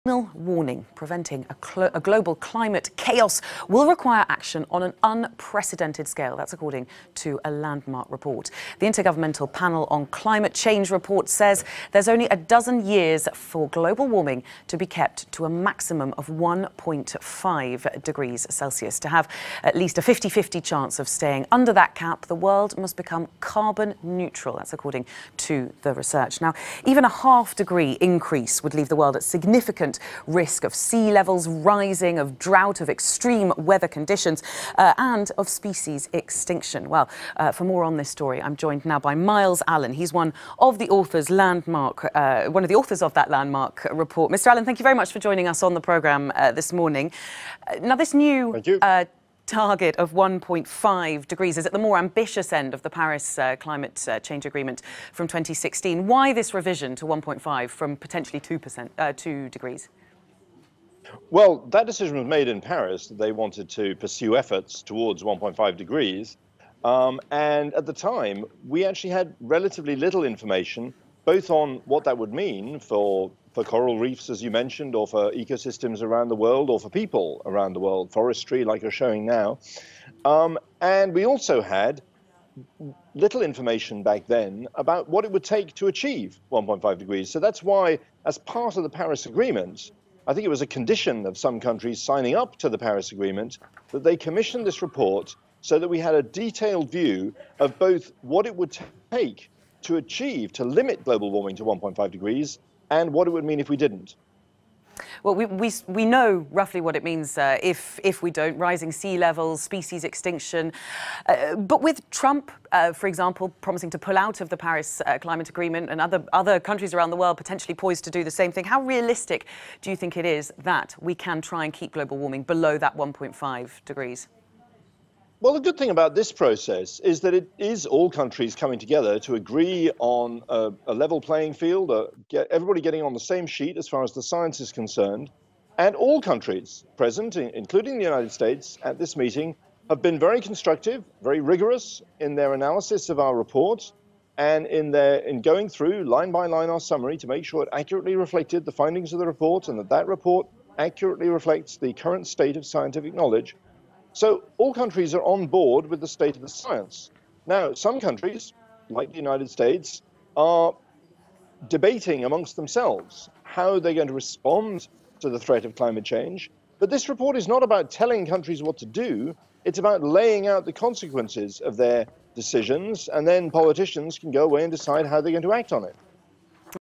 GME | Climate Change: Unprecedented action needed to halt temperature rise
Avoiding global climate chaos will require a transformation of society that is "unprecedented in scale," the UN said in a landmark report, warning that time is running out to avoid disaster.

We talk to Myles Allen one of the authors of the report for more details. What are the top stories today?